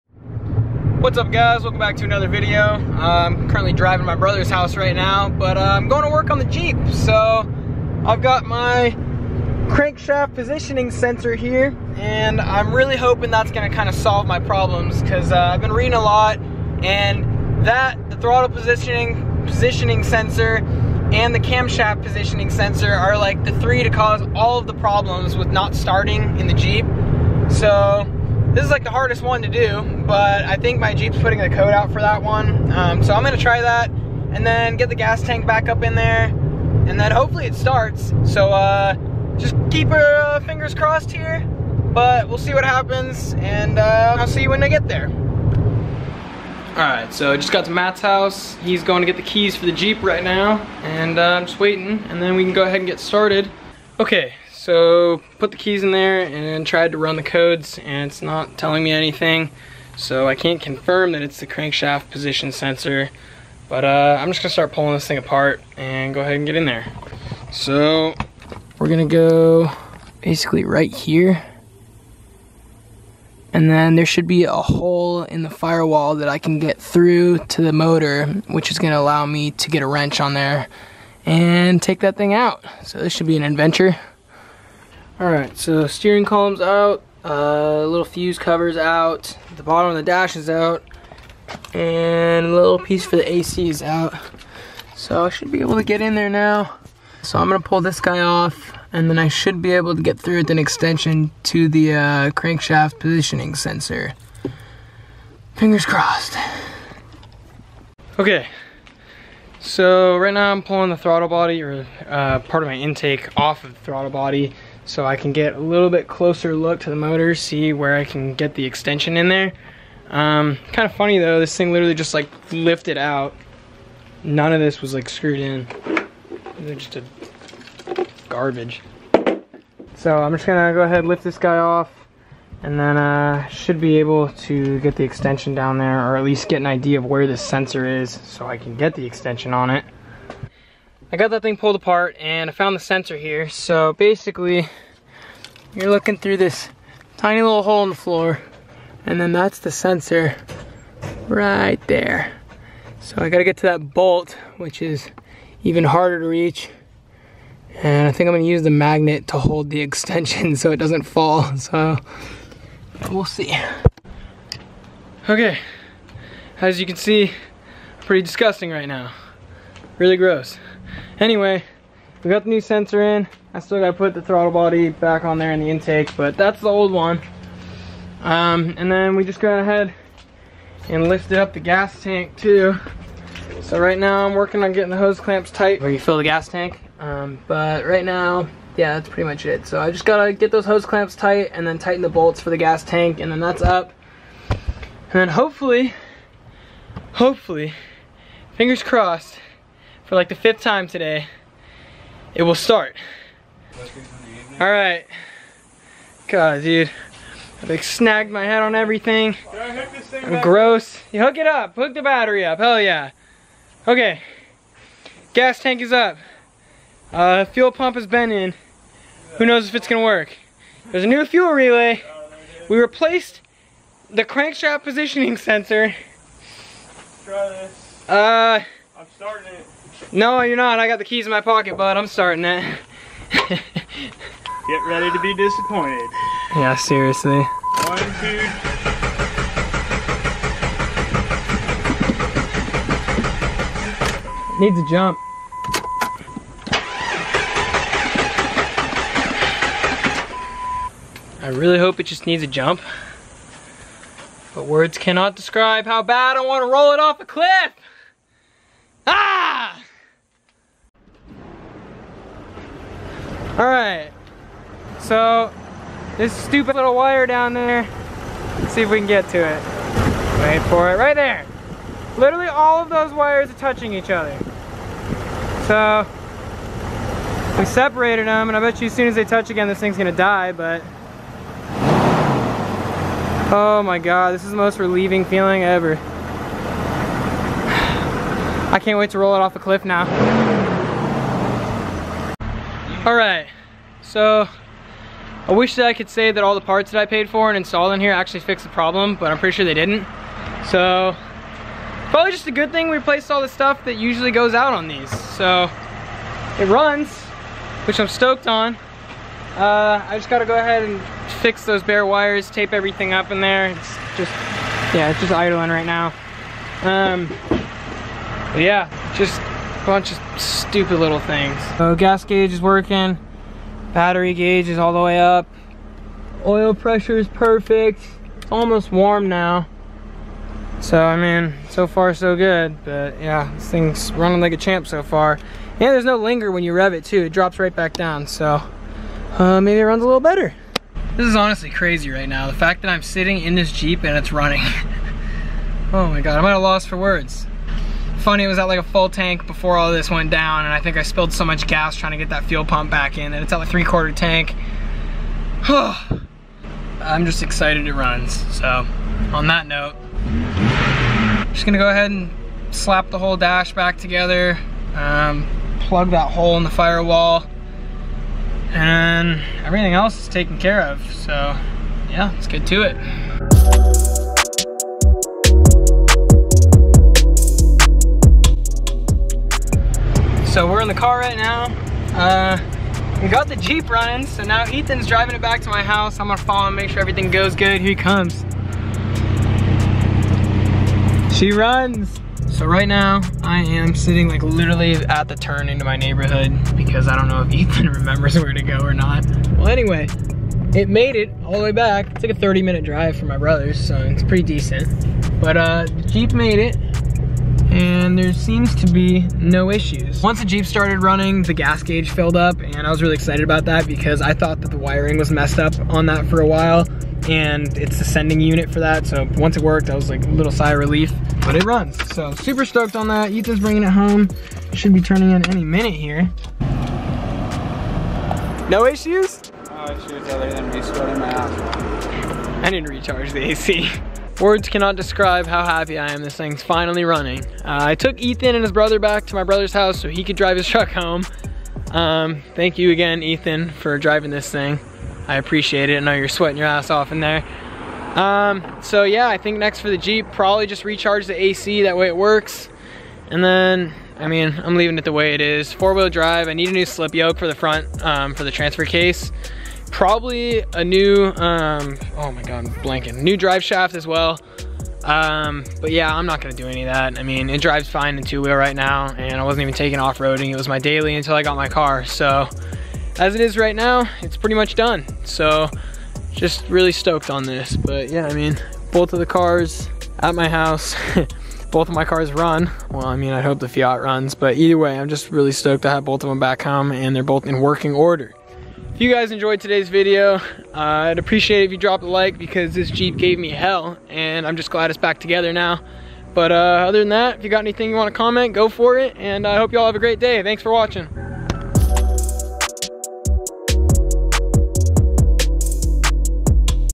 Crankshaft Positioning Sensor | 1999 Jeep Grand Cherokee 4.0L
Finally after hours of troubleshooting, replacing parts, and hitting my head against things, IT RUNS!  I'm pretty excited to get a few little things taken care of and find this thing a new owner, but I have to say it runs better than ever before.  It's been a week with no issues and for a 99' Jeep I'd say thats a win haha. I seriously can't thank my brother enough for giving me a place to store this thing in its time of need, as well as the countless hours that him and my dad helped me research, order parts and work on it.  You guys are life savers and I appreciate all of it!